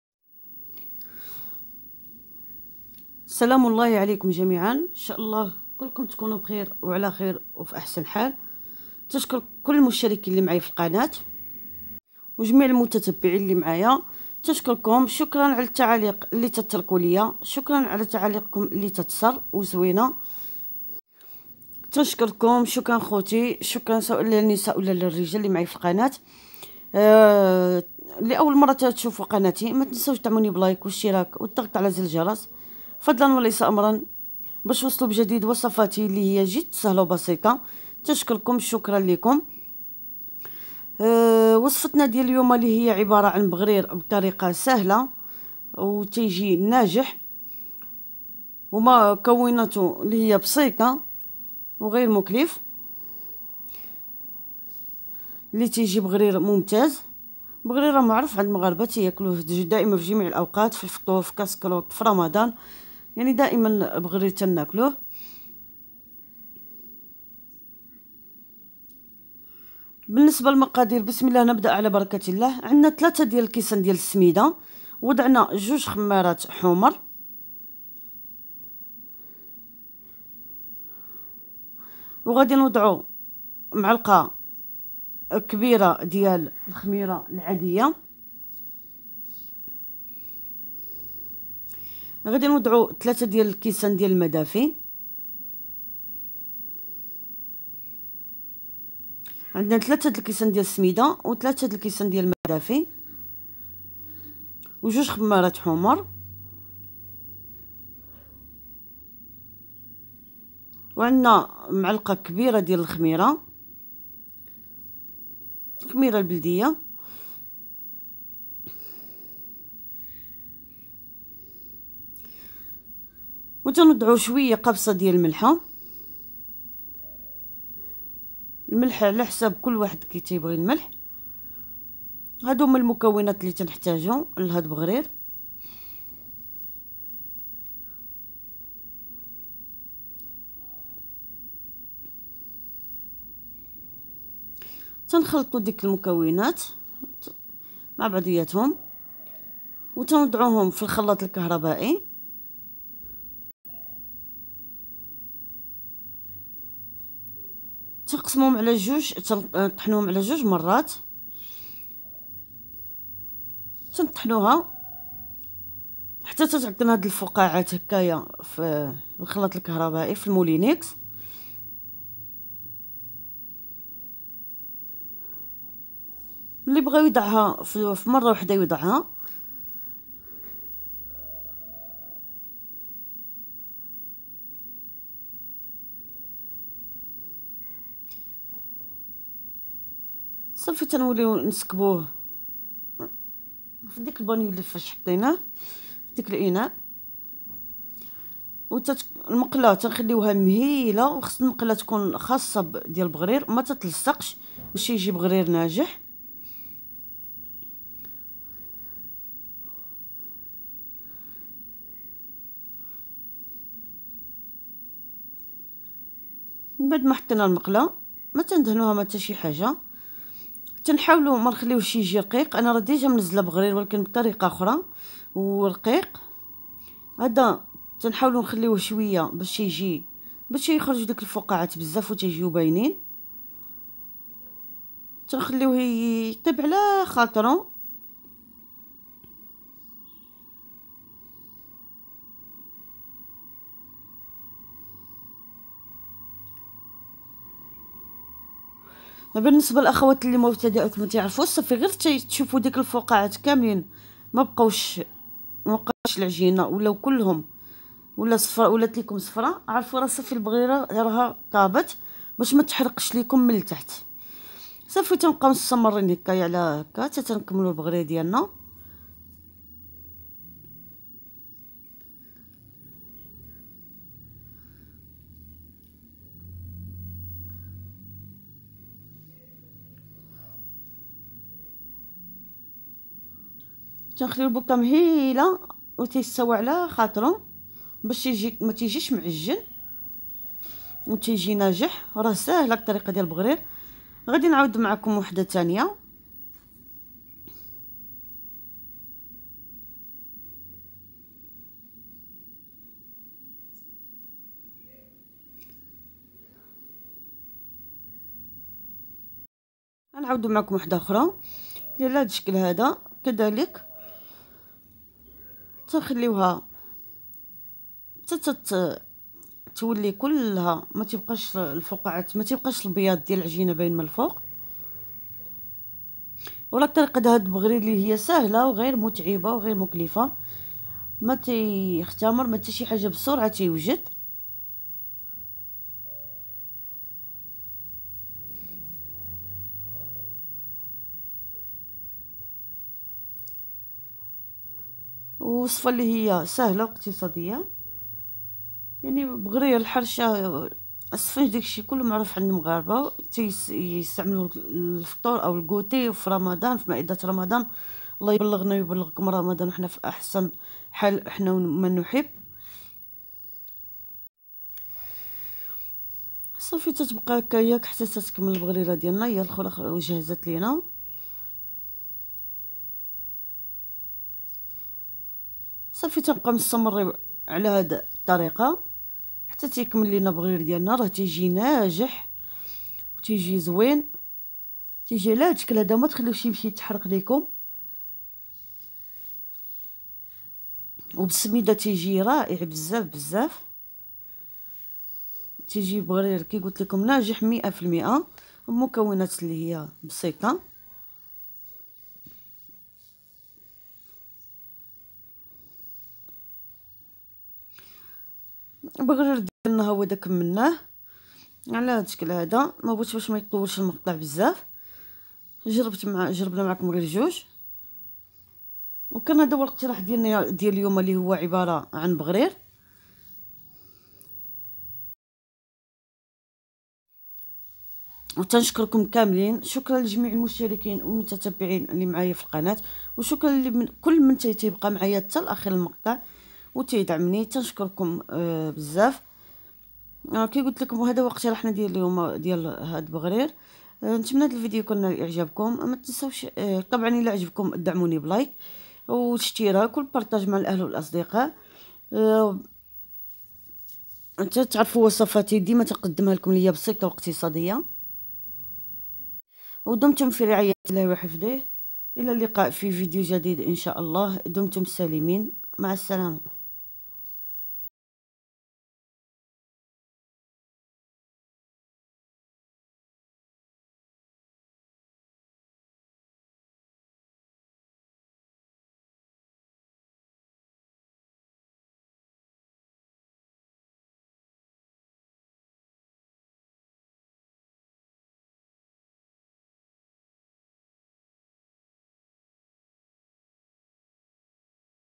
بغرير بطريقة سهلة ومبسطة ناجح من اول مرة
بغرير مغربي

لاتنسوا الإشتراك والشير مع الأهل والأصدقاء ليصلكم كل جديد فضلا وليس أمرا